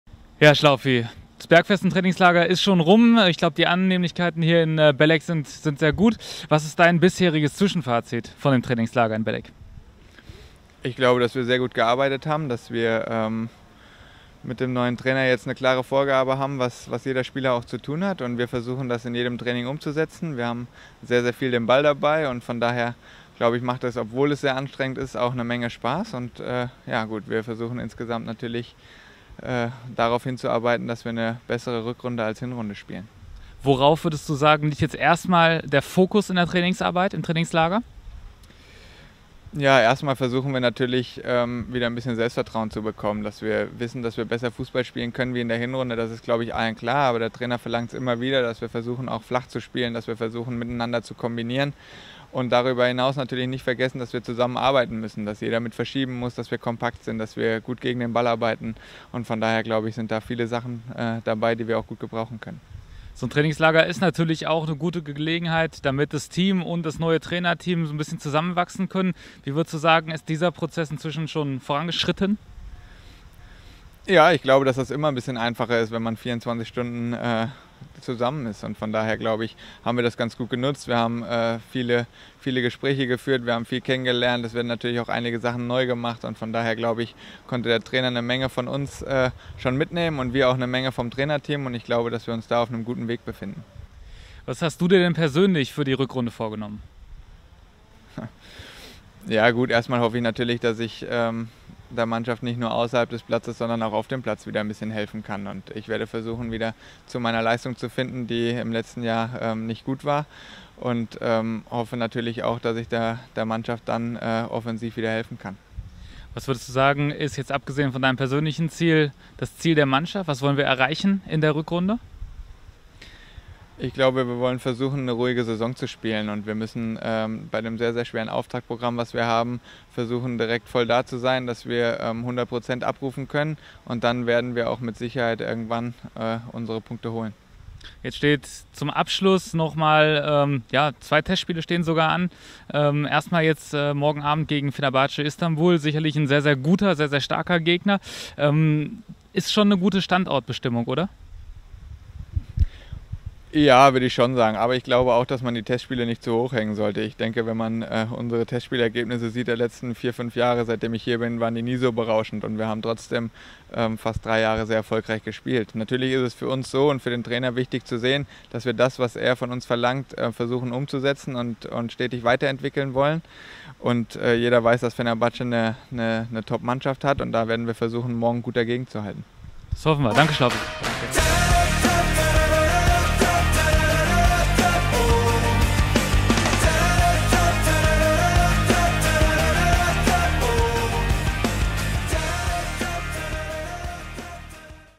Jan Schlaufdraff über das Trainingslager, seine Ziele und das Fenerbahce-Spiel
Am drittletzten Tag des 96-Trainingslagers sprach Kreativdirektor Jan Schlaudraff im Interview über die Einheiten und Eindrücke in Belek, seine persönlichen Ziele für die Rückrunde und das anstehende Testspiel gegen den türkischen Topklub Fenerbahce...